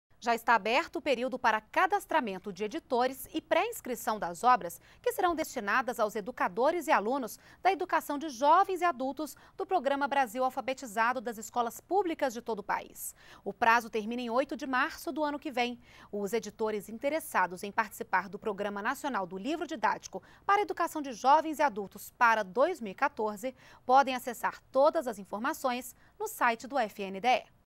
Aberto prazo de cadastramento de editores e obras para Educação de Jovens e Adultos
NBR NOTÍCIAS - 09.08.12: Está aberto o período para cadastramento de editores e pré-inscrição das obras que serão destinadas aos educadores e alunos da Educação de Jovens e Adultos do Programa Brasil Alfabetizado, das escolas públicas de todo o país. O prazo termina dia 8 de março de 2013. Os editores interessados em participar do Programa Nacional do Livro Didático para Educação de Jovens e Adultos, para 2014, podem acessar todas as informações no site do FNDE, no